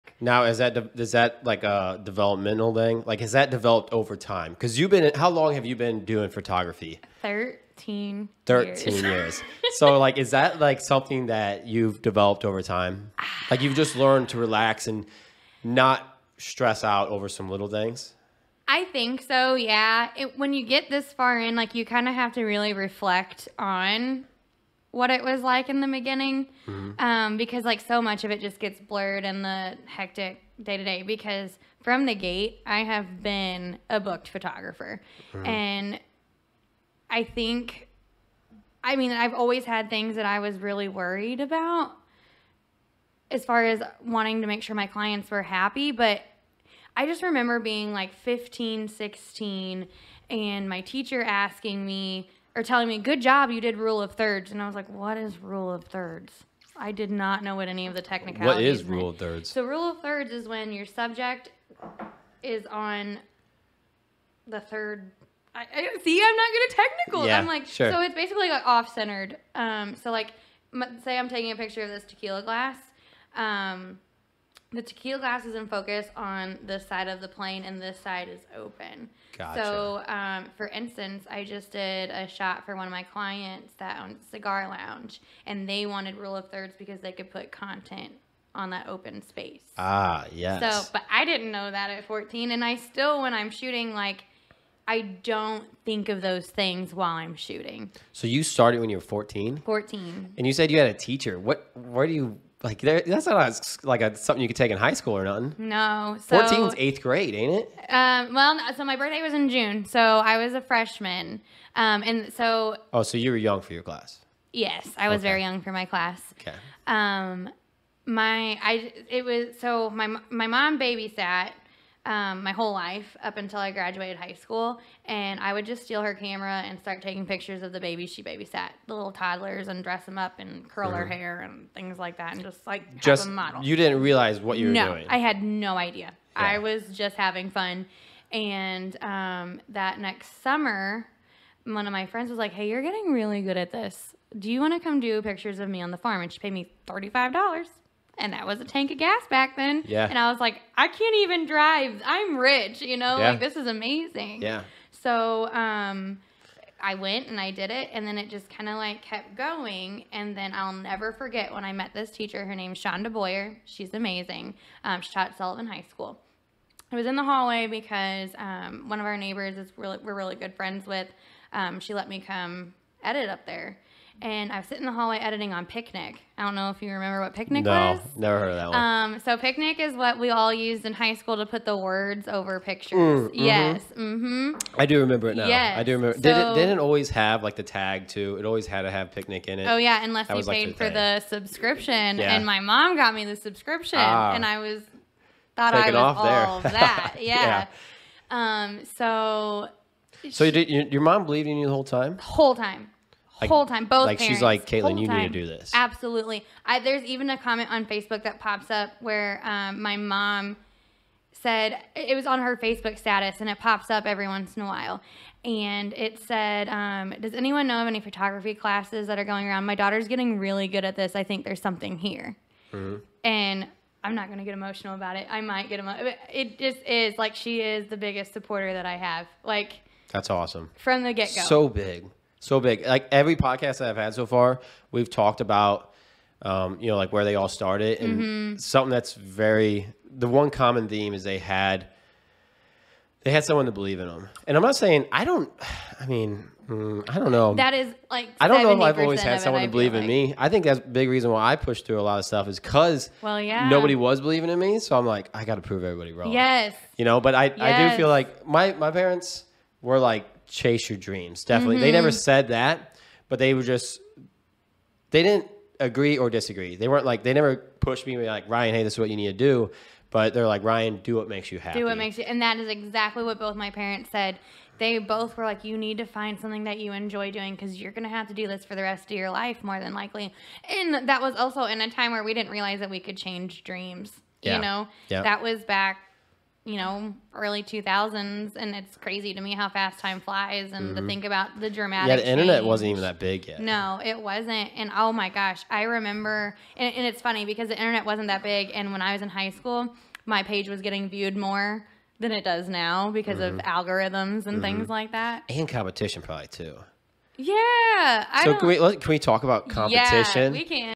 Starting a Photography Business at 14 years old | Ryan Wilson Podcast Clips
Hi everyone, this is a clip from the Kaitlin Mace episode on the Ryan Wilson Podcast, she talks about how she started her photography business at 14 and all the learning that she had to do to get the business to take off and succeed. Make sure to jump over and watch the full podcast to learn a lot more about this fantastic person and all her passions